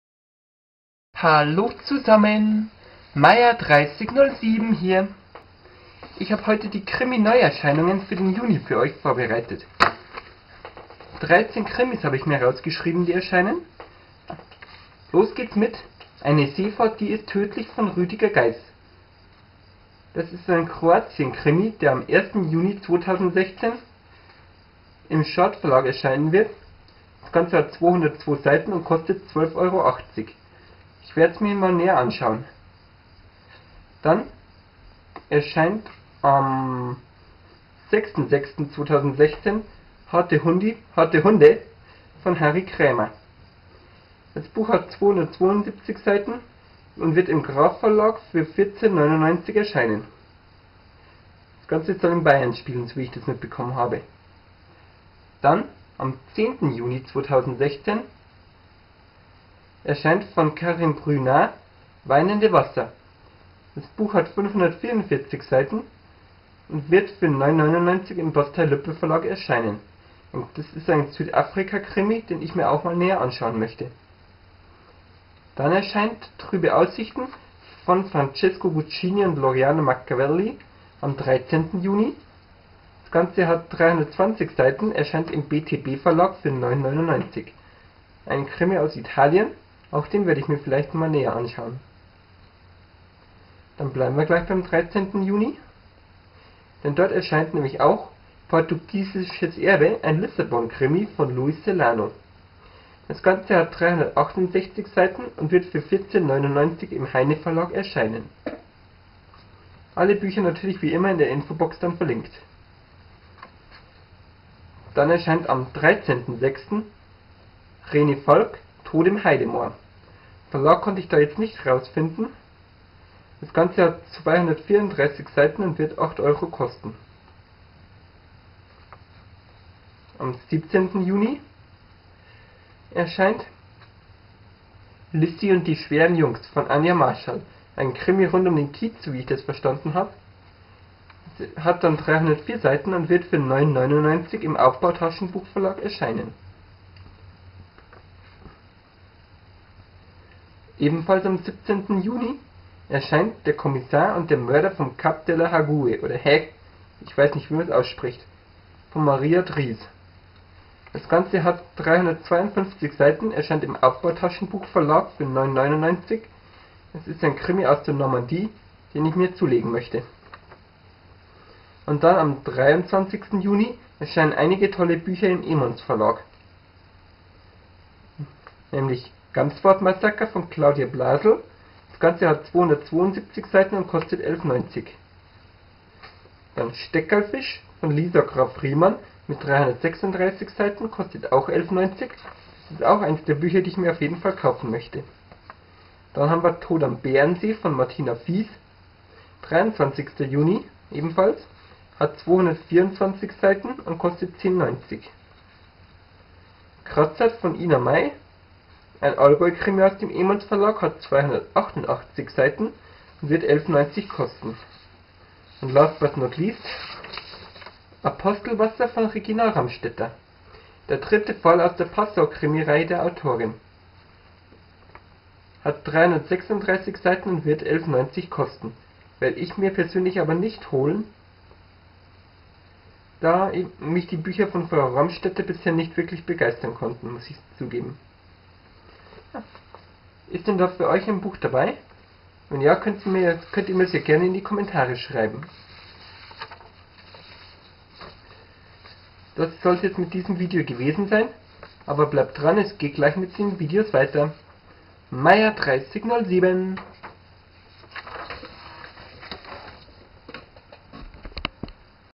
Krimi-Neuerscheinungen Juni 2016
Eine Seefahrt die ist Tödlich

Harte Hunde

Weinende Wasser

Trübe Aussichten

Portugiesisches Erbe

Tod im Heidemoor

Lizzi und die schweren Jungs

Der Kommissar und der Mörder vom Cap de la Hague

Tod am Bärensee